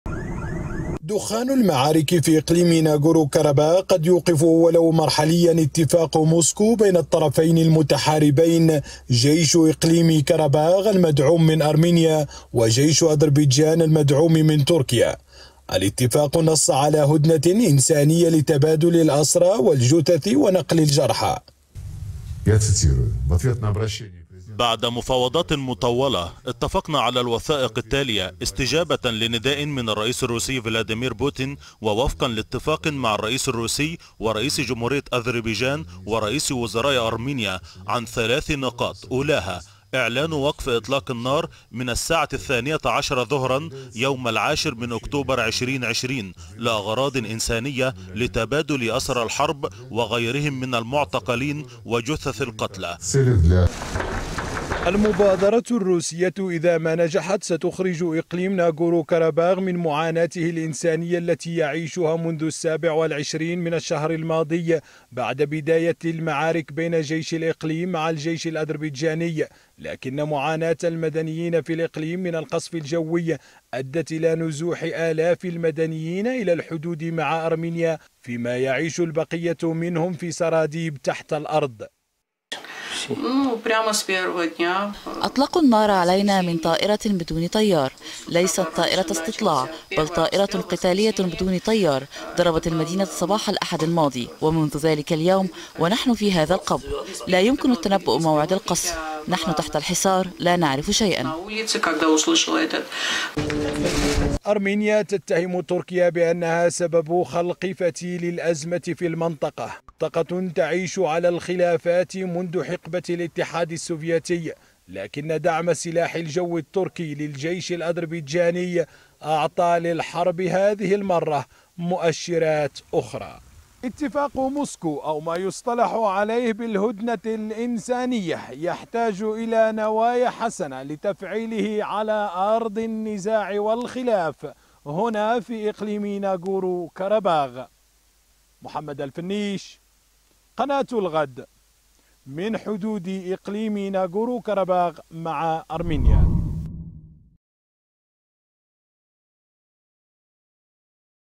بدء هدنة أرمينيا وأذربيجان.. لكنها تحتاج إلى نوايا حسنة لتفعيلها
دخل وقف إطلاق النار في إقليم ناجورنو كاراباخ حيز التنفيذ، وجاء ذلك بعدما شنت كل من أرمينيا وأذربيجان اتهامات متبادلة بالقصف بينهما قبل سريان وقف إطلاق النار.
وكان وزيـر الخارجية الروسي سيرغي لافروف قد أعلن التوصل لاتفاقٍ لوقف إطلاق النار, بين أرمينيا وأذربيجان, بدءا من اليوم .. وأضح لافروف أن وزيـري خارجية البلدين اتفقا خلال اجتماعهـما في موسكو على تبادل الأسرى وجثث القتلى خلال الهدنة.
المزيد من التفاصيل في تقرير مراسل الغد من على الحدود الآرمينية وكاراباخ محمد الفنيش.